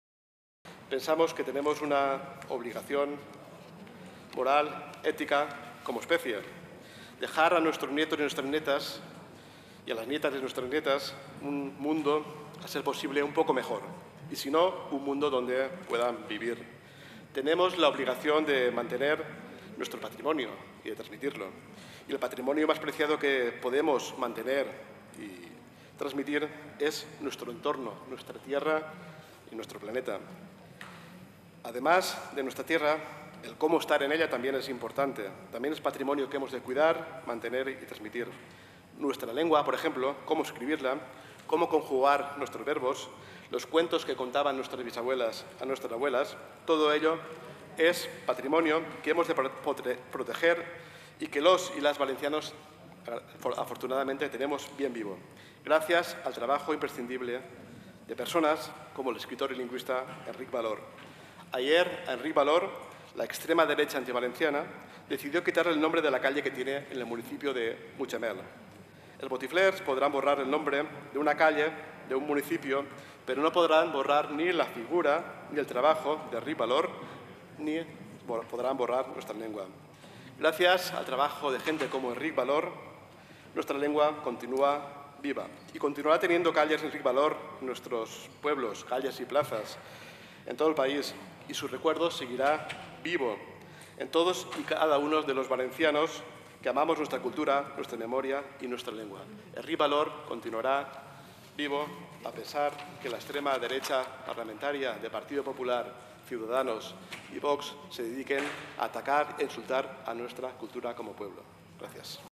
✏️Mulet: "Els botiflers podran esborrar el nom d’un carrer, però no la figura d’Enric Valor"
Intervenció de Carles Mulet, Portaveu de Compromís al Senat, en defensa de la figura d'Enric Valor, a qui la dreta de PP, Ciudadanos i Vox volen llevar-li el carrer a Mutxamel per a posar-li de nom "Avenida de España".